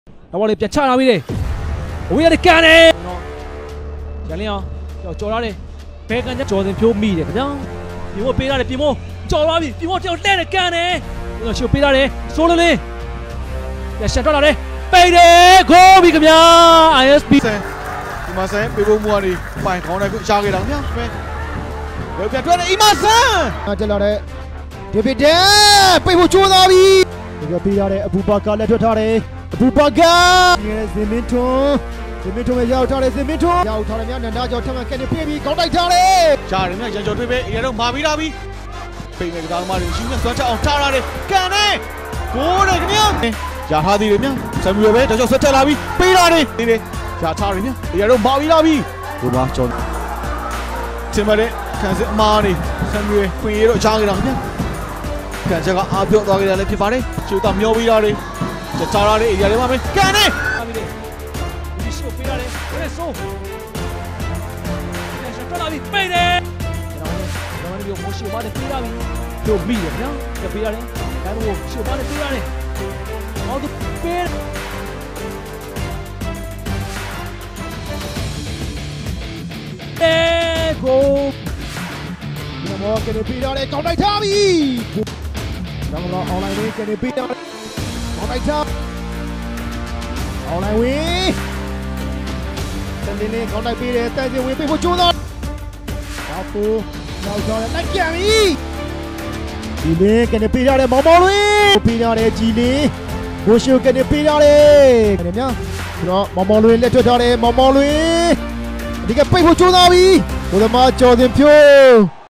Kyaw Zin Phyo (Clean Sheet) Ayeyawady United
MPT Myanmar National League 2020 အမှတ်ပေးဘောလုံးပြိုင်ပွဲ ပွဲစဉ် ကိုးပွဲစီ ကစားအပြီးမှာ ဧရာဝတီယူနိုက်တက်အသင်း ဂိုးသမား ကျော်ဇင်ဖြိုးက ခြောက်ပွဲ Clean Sheet ရခဲ့ပြီး ပေးဂိုးကလဲ သုံးဂိုးသာ ရှိခဲ့ပါတယ်. ။

ဧရာဝတီယူနိုက်တက်အသင်း ဂိုးသမား ကျော်ဇင်ဖြိုးရဲ့ လက်စွမ်းပြကာကွယ်မှုအချို့ကို စုစည်းတင်ဆက်ပေးလိုက်ပါတယ်။